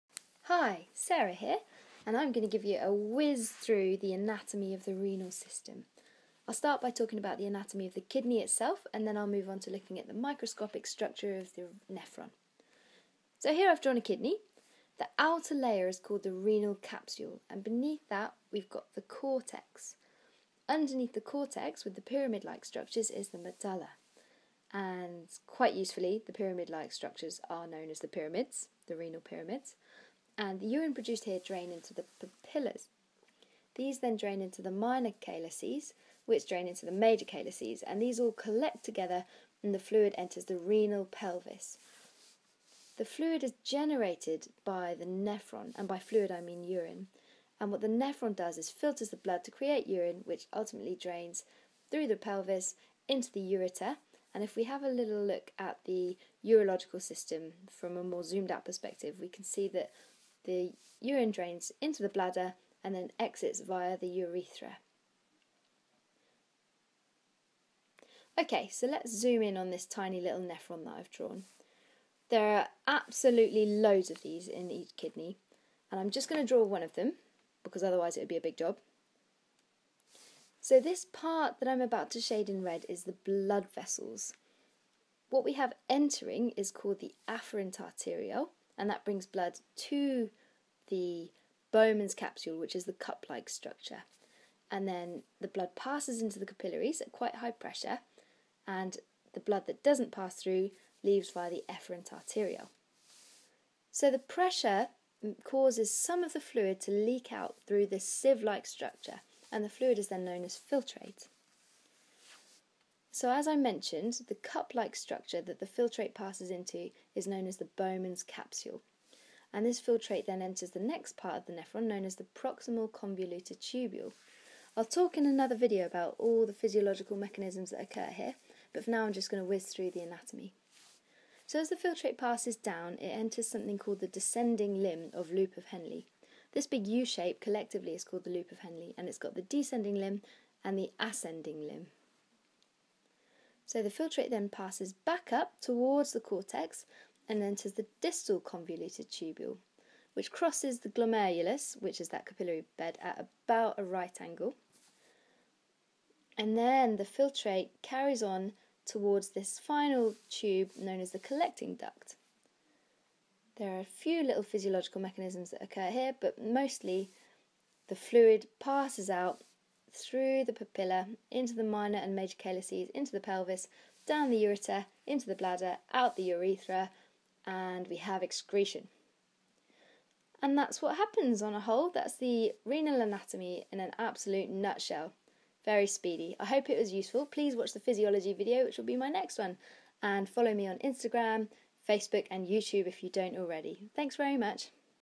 Anatomy of the Kidney and Nephron - Sarah Clifford Illustration Tutorial
This illustrated tutorial gives an overview of the anatomy of the kidney and nephron.